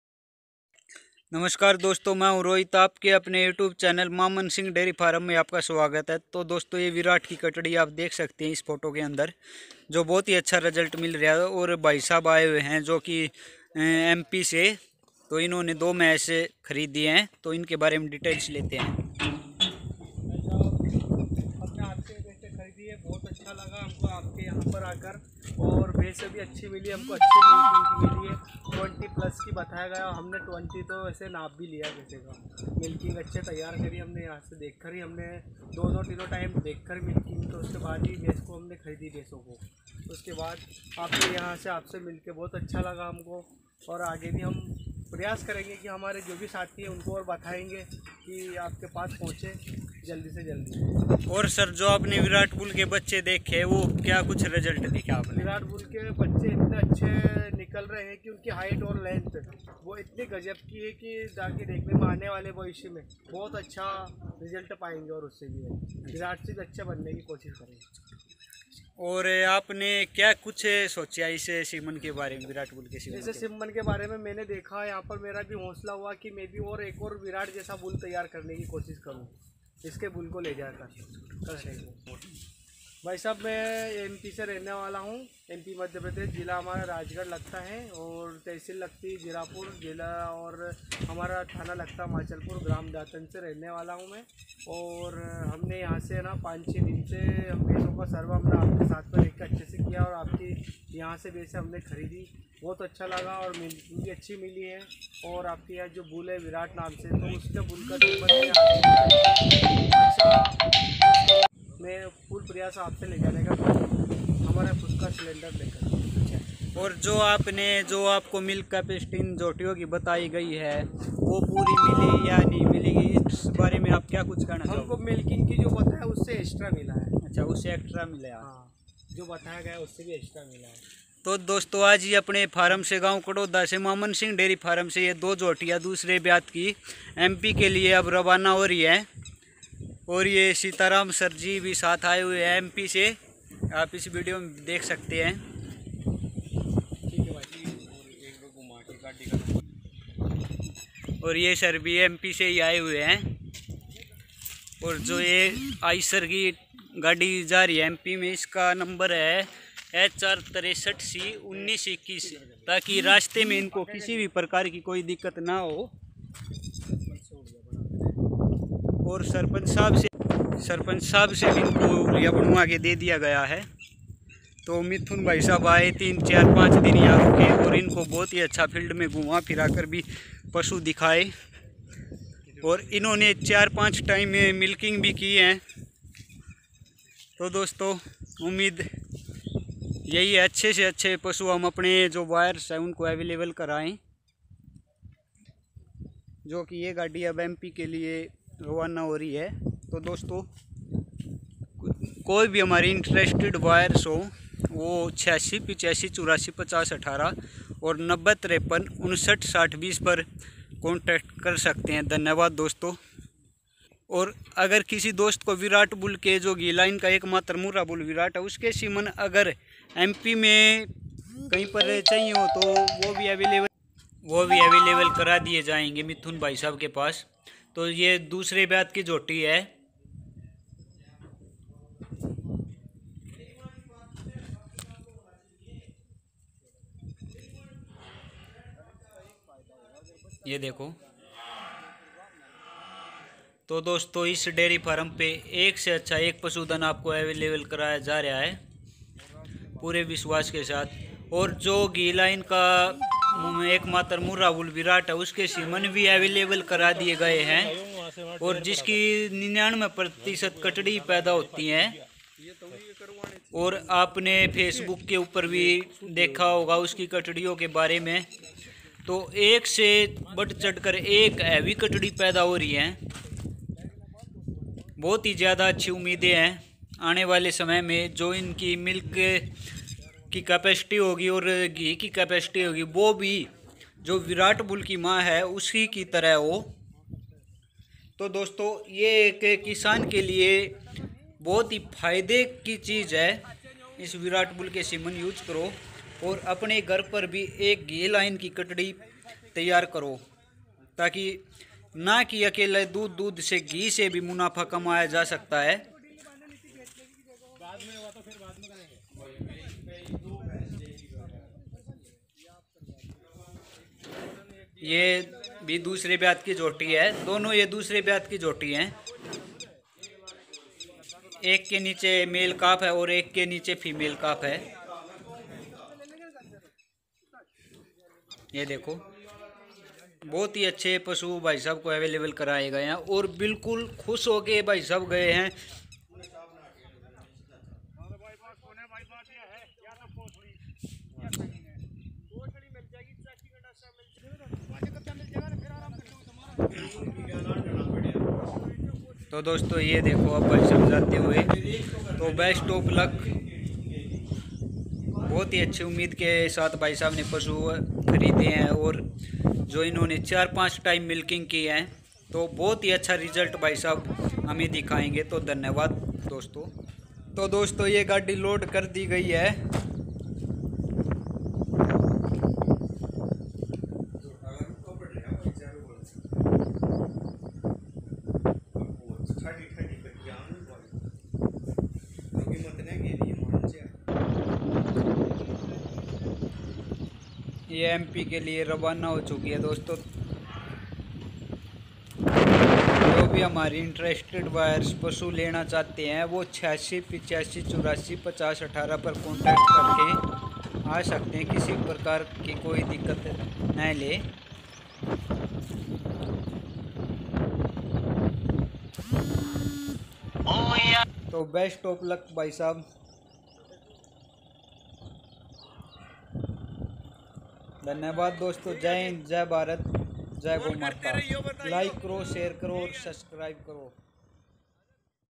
ऐसा क्या है मामन सिंह डेयरी में जो इतना भरोसा करते हैं जो बहार से पशु लेने आते हैं - 8685845018
Village Karodha
Distic Jhajjar
State Haryana